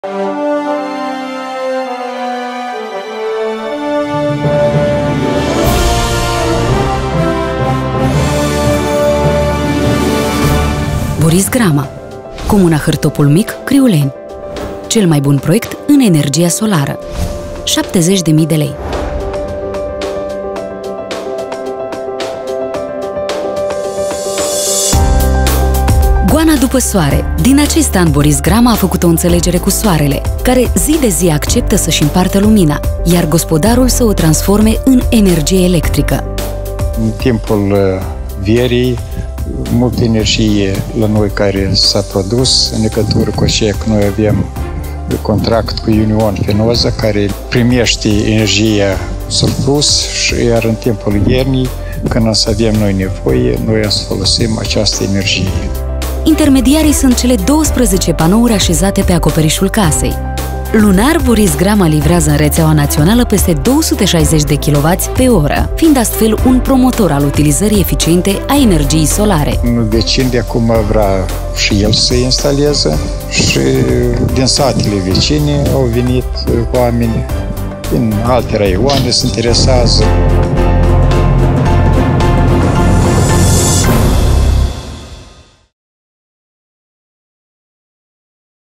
Moldova Eco Energetica 2013, Cel Mai Bun Proiect in Energia Solara
Boris Grama din Hartopul Mic, Criuleni, are energie electrica in casa de la cele 12 panouri fotovoltaice instalate pe acoperisul modestei sale case. In plus la asta, lunar, Boris Grama livrează în reţeaua naţională peste 260 de kWh,  Boris Grama este detinatorul trofeului pentru Cel Mai Bun Proiect in Energia Solara Fotovoltaica, editia 2013, Moldova Eco-Energetica.